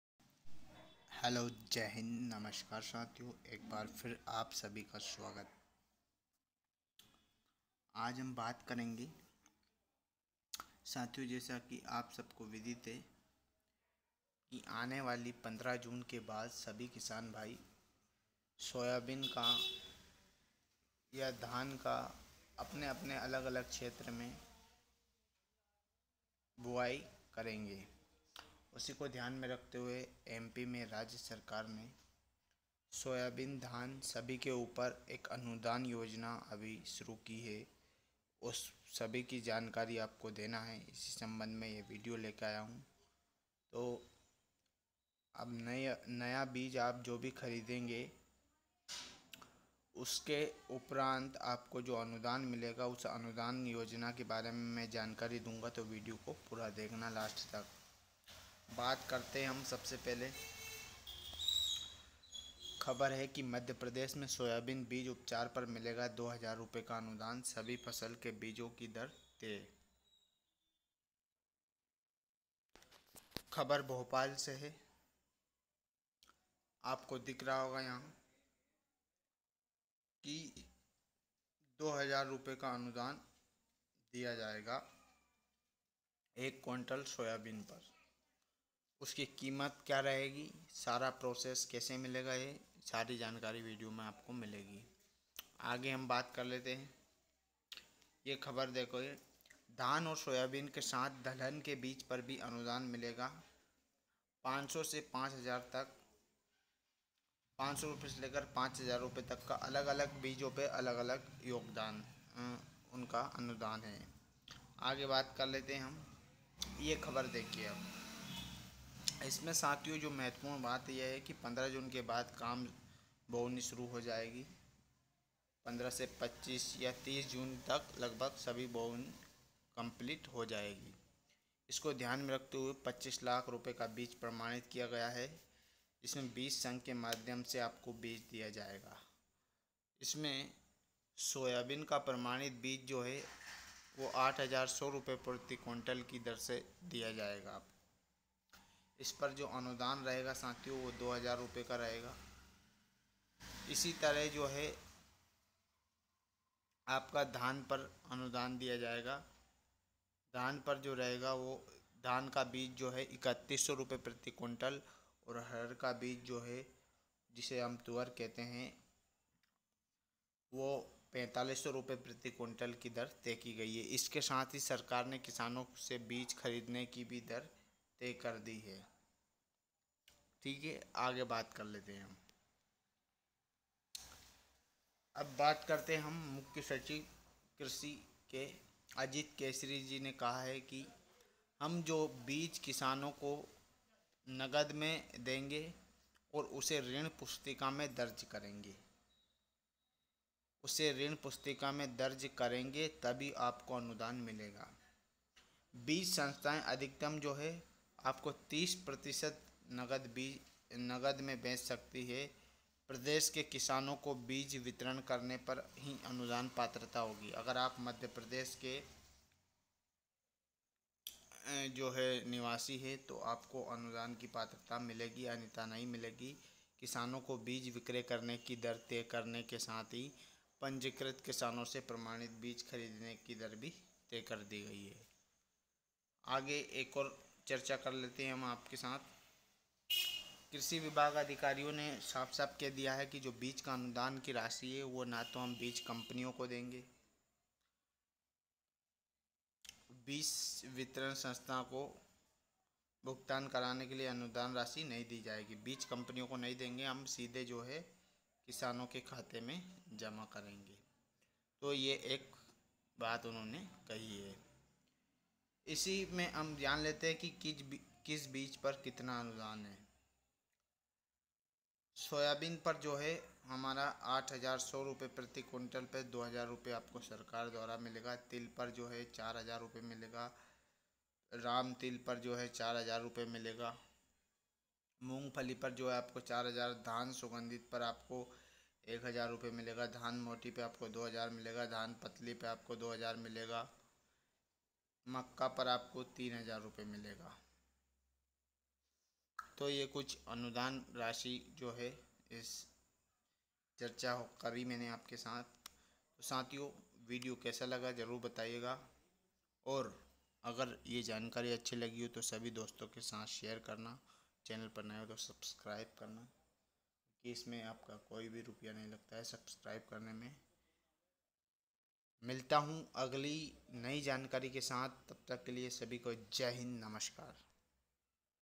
मध्य प्रदेश के सभी किसानों को सोयाबीन धान मक्का तिल आदि बीज खरीदने पर सरकार द्वारा अनुदान दिया जाएगा
मध्‍य प्रदेश में सोयाबीन बीज पर मिलेगा 2000 रुपये का अनुदान, सभी फसलों के बीज की दर तय
8100 रुपये प्रति क्विंटल में मिलेगा सोयाबीन बीज, 3100 रुपये में सुगंधित धान का बीज

Updated: | Thu, 09 Jun 2022 06:58 PM (IST)
naidunia

राज्य सरकार ने प्रति क्विंटल दिए जाने वाले अनुदान की दर भी की तय

यह भी पढ़ेंnextStory
MP Urban Body Elections 2022: 347 नगरीय निकाय चुनाव के लिए शनिवार से जमा होंगे नामांकन पत्र
भोपाल (राज्य ब्यूरो)। मध्य प्रदेश में 15 जून के बाद खरीफ फसलों की बोवनी का काम शुरू हो जाएगा। राज्य सरकार ने 25 लाख क्विंटल प्रमाणित बीज का इंतजाम भी कर लिया है। साथ ही किसानों को सहकारी समिति और बीज संघ के माध्यम से दिए जाने वाले बीजों की दर भी तय कर दी है। सोयाबीन का प्रमाणित बीज 8100 रुपये प्रति क्विंटल की दर से दिया जाएगा। इस पर 2000 रुपये का अनुदान दिया जाएगा। इसी तरह सुगंधित धान के बीज की दर 3100 रुपये प्रति क्विंटल और अरहर (तुअर) के बीज की दर 4500 रुपये प्रति क्विंटल तय की गई है। इसके साथ ही सरकार ने किसानों से बीज खरीदने की दर भी तय कर दी है।

अपर मुख्य सचिव (कृषि) अजीत केसरी ने बताया कि किसानों को जो भी बीज नकद में दिया जाएगा, उसे ऋण पुस्तिका में दर्ज करना होगा। बीज संस्थाएं अधिकतम 30 प्रतिशत बीज नकद में बेच सकती हैं। प्रदेश के किसानों को ही बीज वितरण करने पर अनुदान की पात्रता मिलेगी। किसानों को बीज विक्रय करने की दर तय करने के साथ ही पंजीकृत किसानों से प्रमाणित बीज खरीदने की दर भी तय कर दी गई है।